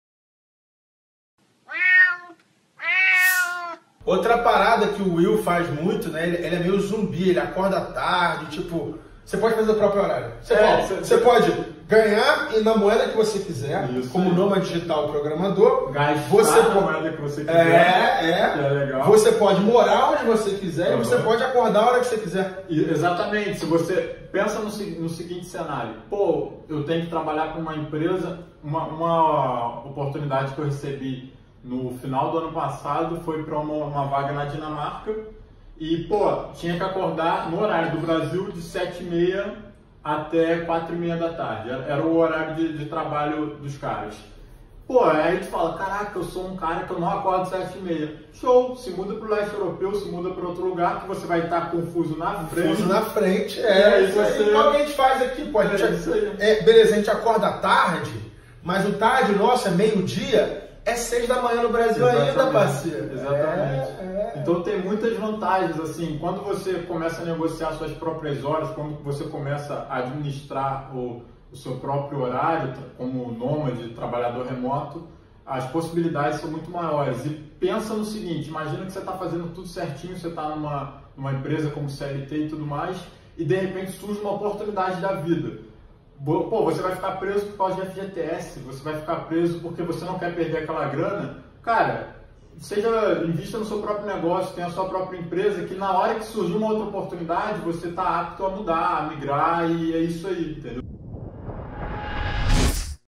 ESSE É O MELHOR TRABALHO DO MUNDO - PULO DO GATO #6
Fala rapaziada, já parou para pensar qual é o melhor trabalho do mundo? Será que você também pode ter ele? Vou te mandar a real, com ele você pode trabalhar onde quiser, quando quiser e ainda ganhar em dólar, então confere ai!

QUER FALAR COMIGO DIRETAMENTE?

ESSE É O MELHOR TRABALHO DO MUNDO